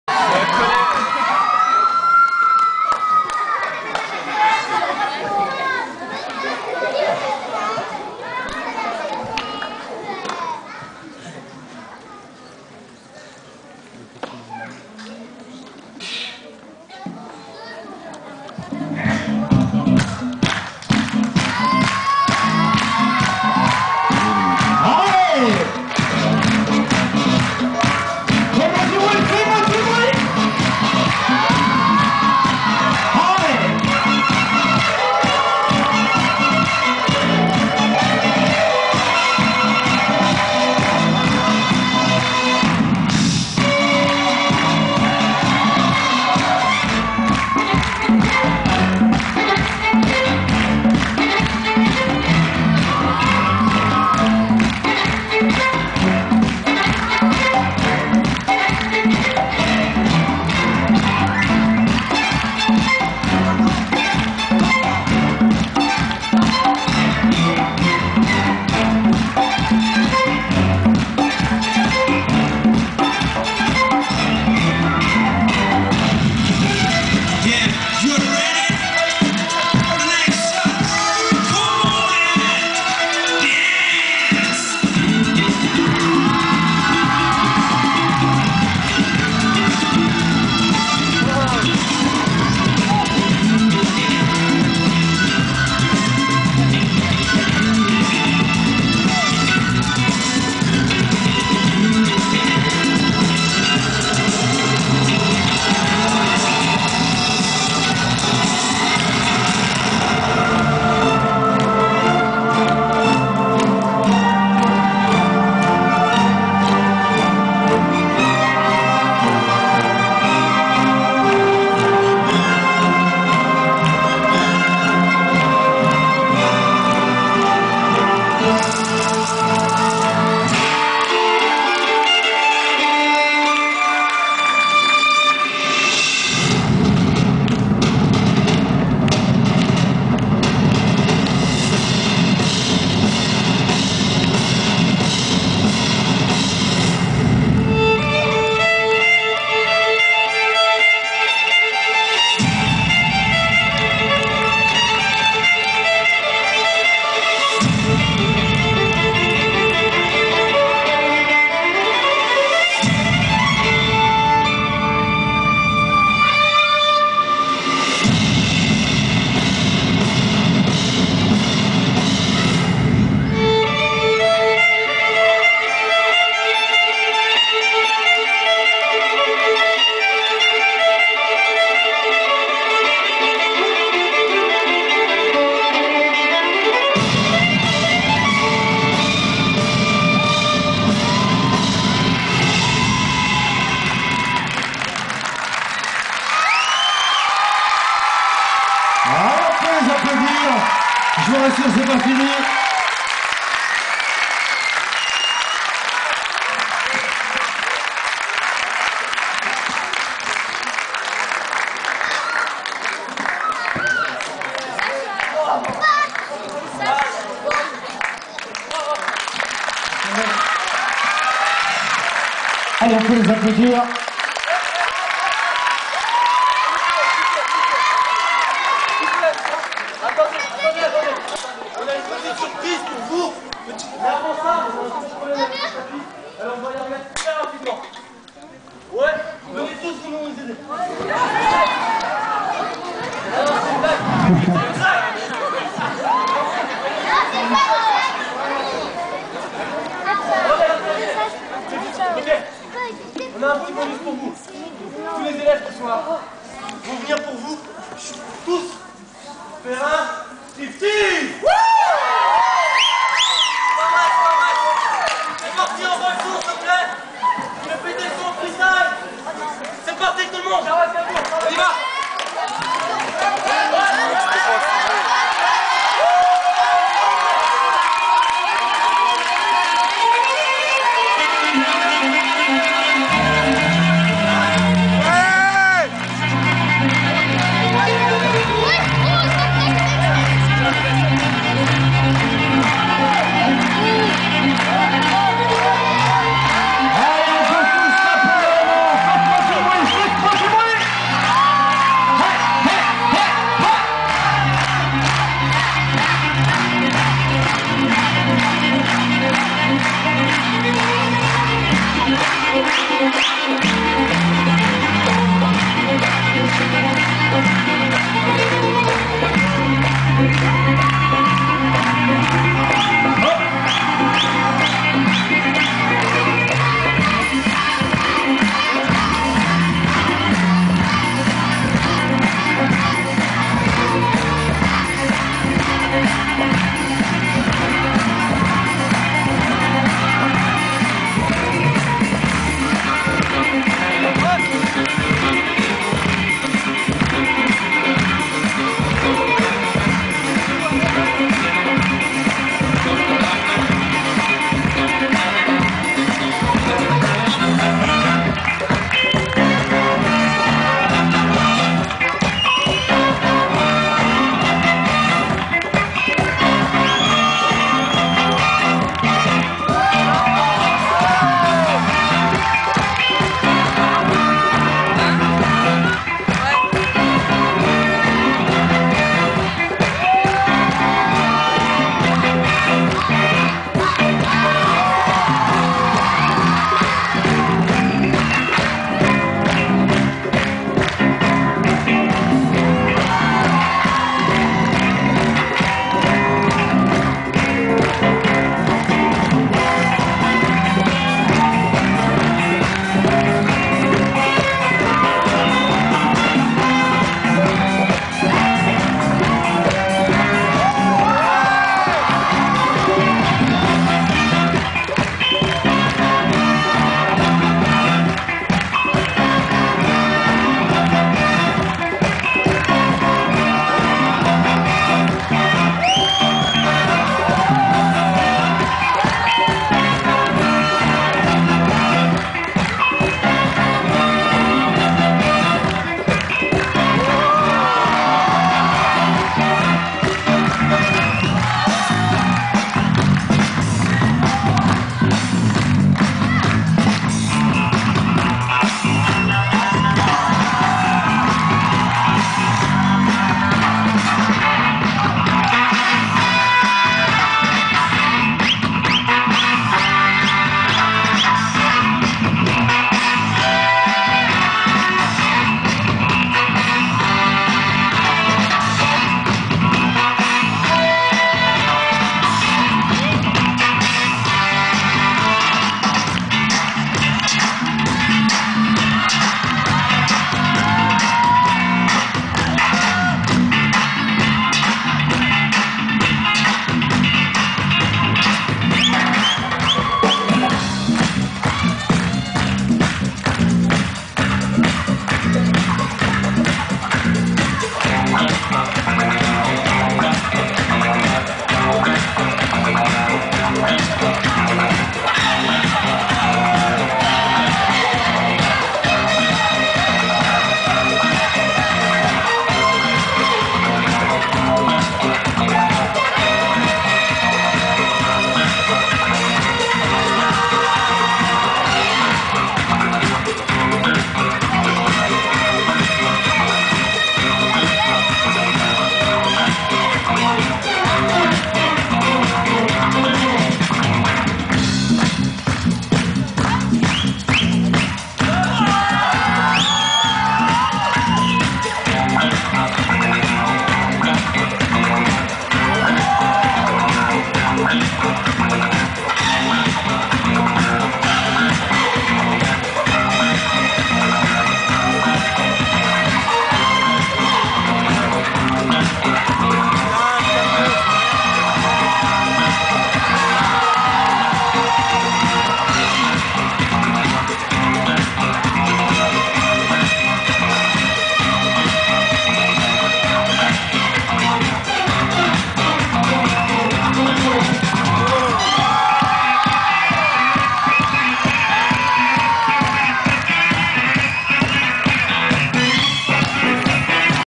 soirée de gala de breakdance le 14 juin
soirée de gala de breakdance le 14 juin à fleurieux pour vivi